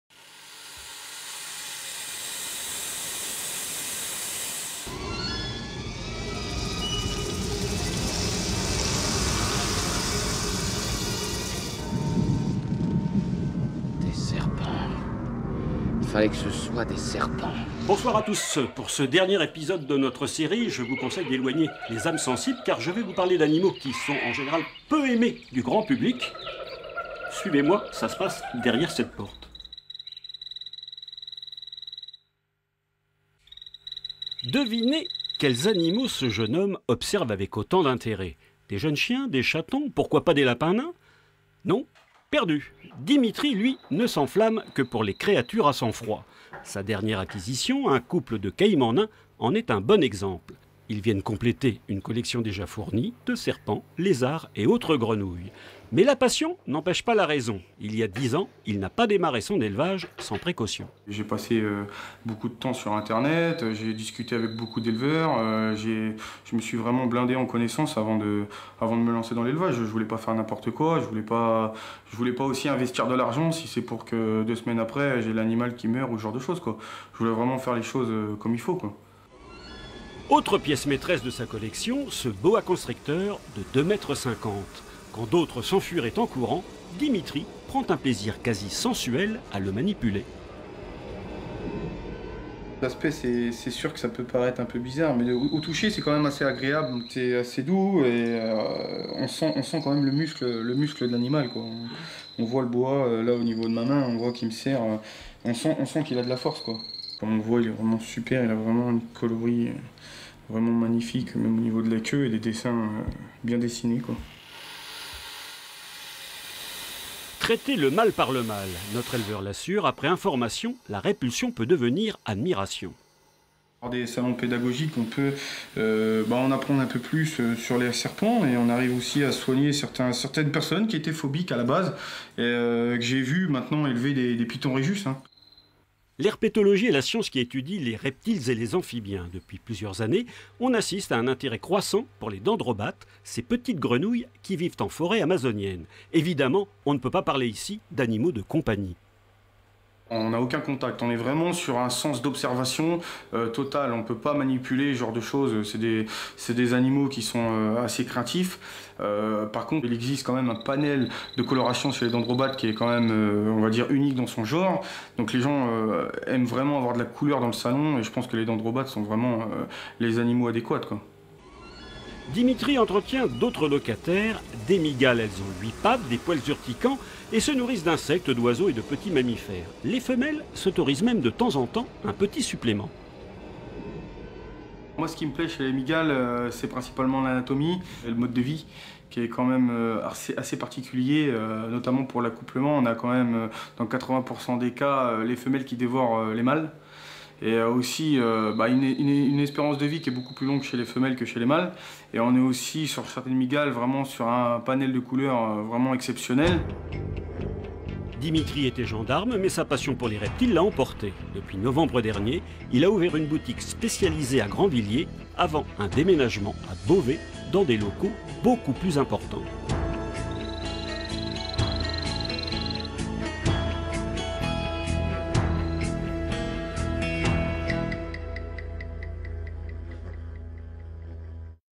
La passion des reptiles de Dimitri
Dimitri Ferre est passionné de reptiles. Caïmans nains, serpents, lézards, grenouilles, sa collection est bien fourni. Si on ne peut pas parler d'animaux de compagnie, l'intérêt pour les reptiles et les amphibiens est croissant. Si bien que Dimitri compte déménager sa boutique spécialisée dans de meilleurs locaux à Beauvais.